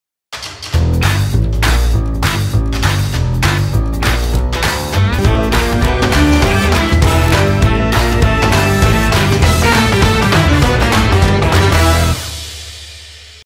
Вигравай календар "2020. Твій успішний рік!"
👋Увага!!! Розігруємо 3 (три) календарі "2020. ТВІЙ УСПІШНИЙ РІК" від Divaki production.
Хто не встиг почати рік з 1 січня, є нагода розпочати його зі старого Нового Року🎉. І отримати в подарунок наш календар з мотиваційними фразами на кожен місяць від диваків.

Умови участі прості:
2. Поставити 👍 цьому відео .
3. Написати коментар під цим відео.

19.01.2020 р. ми визначимо переможців за допомогою Random.org. Твій Успішний рік розпочався!!!😉

Youtube не несе відповідальності перед учасниками розіграшу. і жодним чином не фінансує, не санкціонує й не адмініструє розіграш та ніяк із ним не пов’язана.